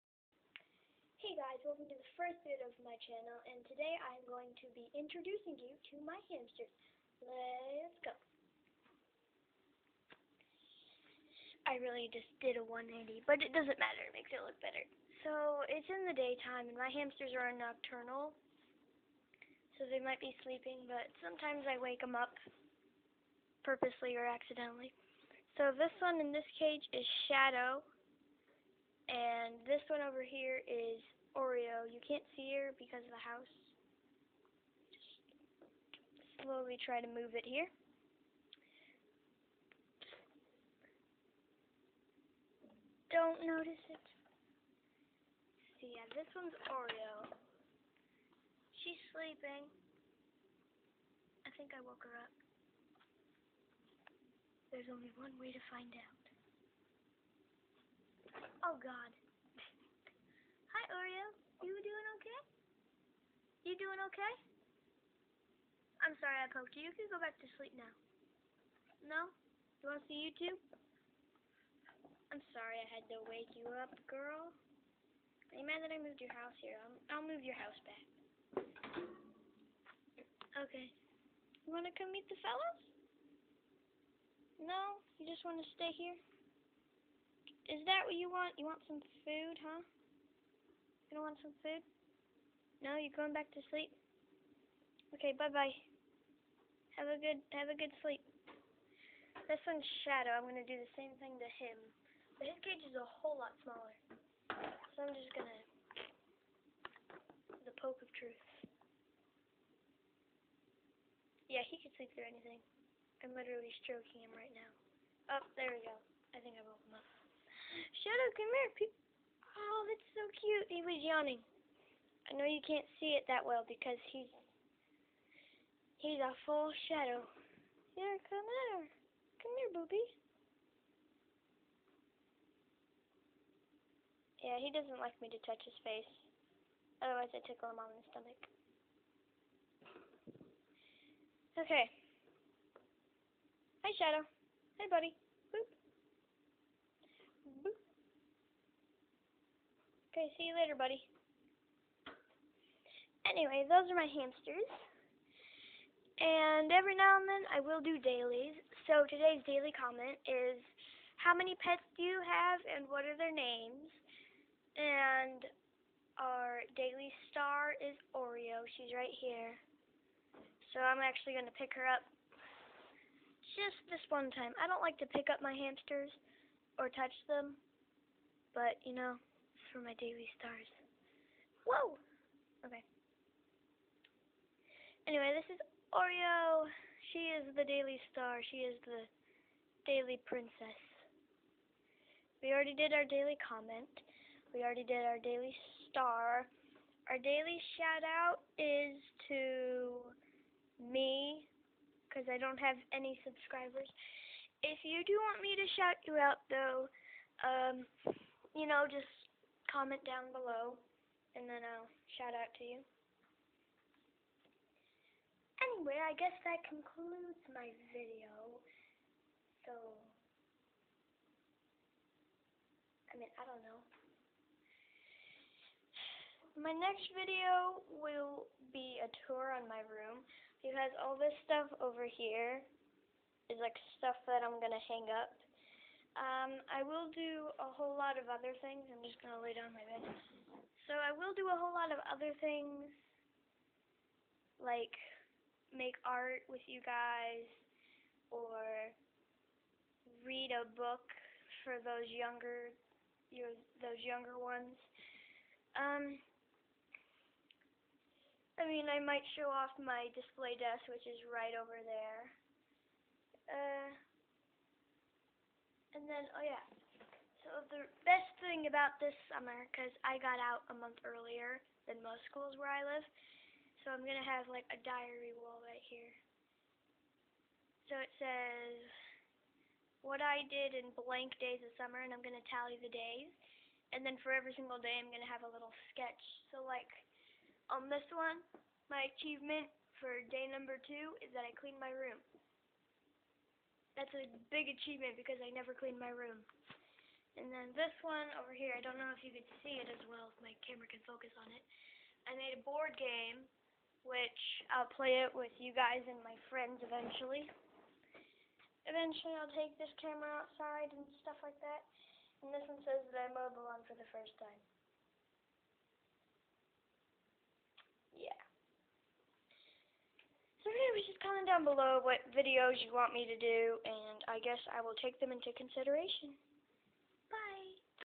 Introducing My Hamsters!
Welcome to my very first video of this channel! I will be doing a variety of other videos, so just tell me what you want to watch. :)